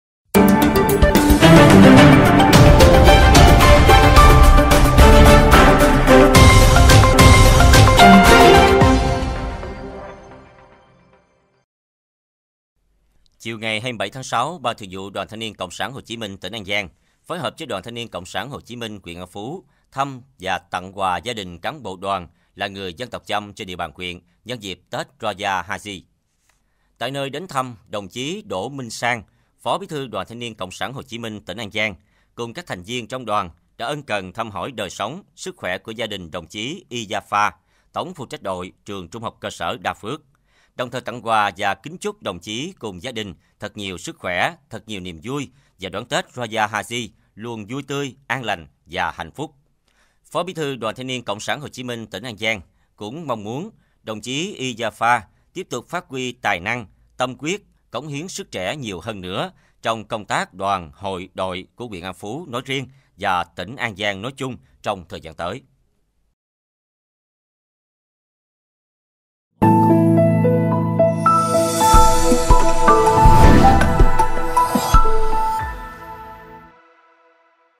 Tỉnh đoàn An Giang thăm và tặng quà cho cán bộ Đoàn là đồng bào dân tộc Chăm nhân dịp Tết Roya Haji
Đoàn TNCS Hồ Chí Minh tỉnh An Giang thăm và tặng quà cho cán bộ Đoàn là đồng bào dân tộc Chăm nhân dịp Tết Roya Haji